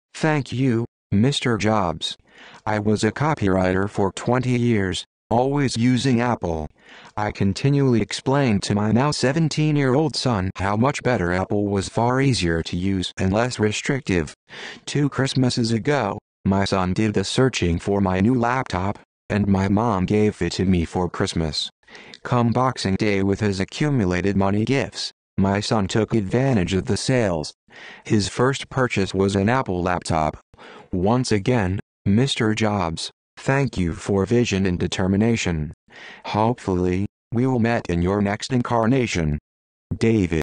Remembering Steve #756
Thank you, Mr. Jobs
I was a copywriter for 20 yrs, always using Apple. I continually explained to my now 17-year-old son how much better Apple was far easier to use and less restrictive. Two Christmases ago, my son did the searching for my new laptop, and my Mom gave it to me for Christmas. Come Boxing Day with his accumulated money gifts, my son took advantage of the sales. His first purchase was an Apple laptop. Once again, Mr. Jobs, thank you for vision and determination. Hopefully, we'll met in your next incarnation.
David